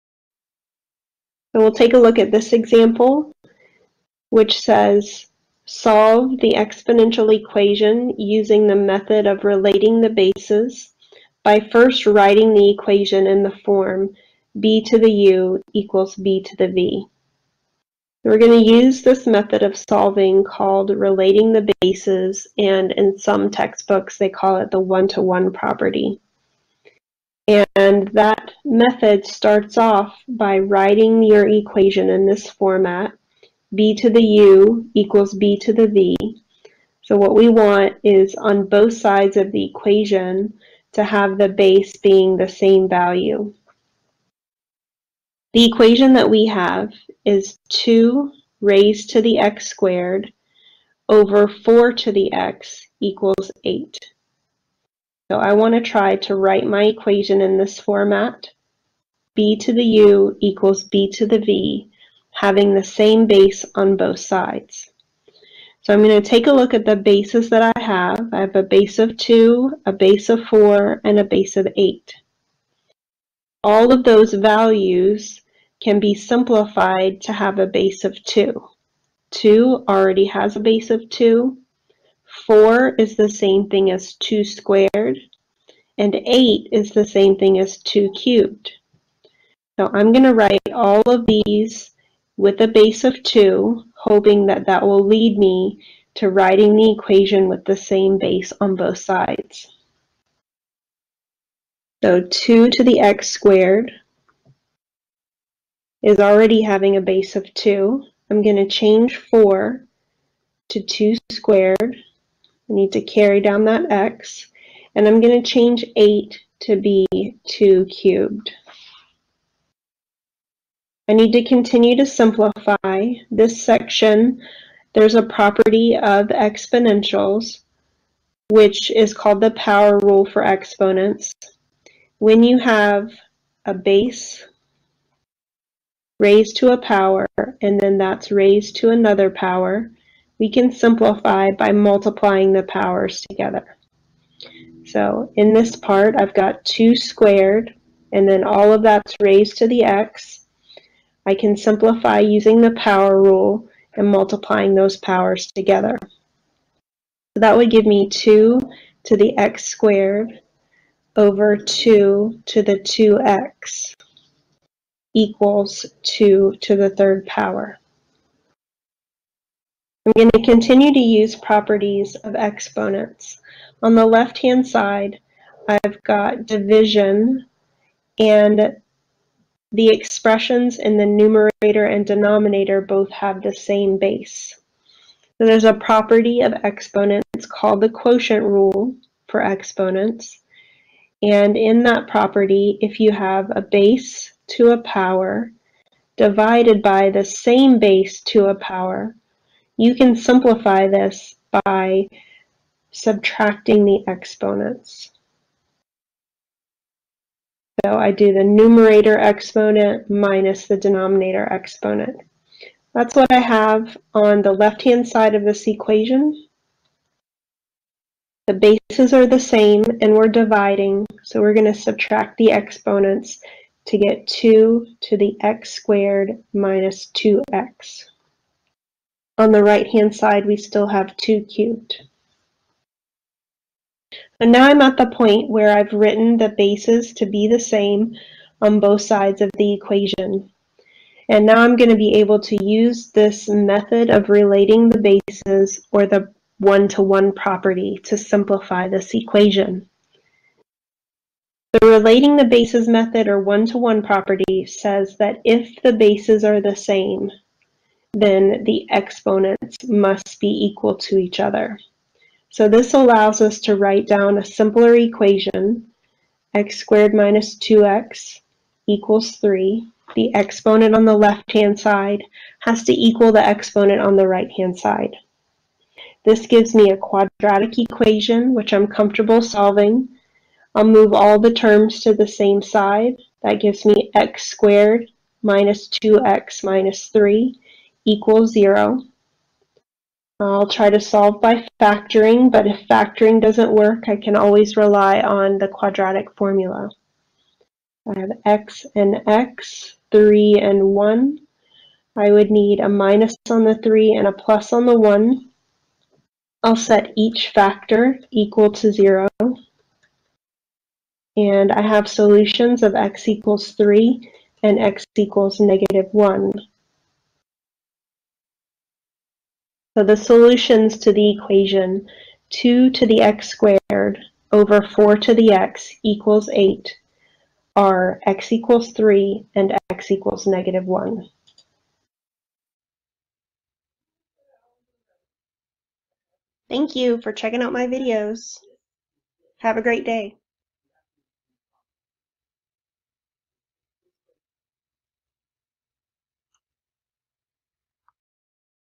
Solve an Exponential Equation by Relating the Bases
Solve the exponential equation by relating the bases otherwise know as the one to one property.  You will also use one of the properties of exponents called the power rule.

Website Posts on Solving Exponential Equations by Making the Bases the Same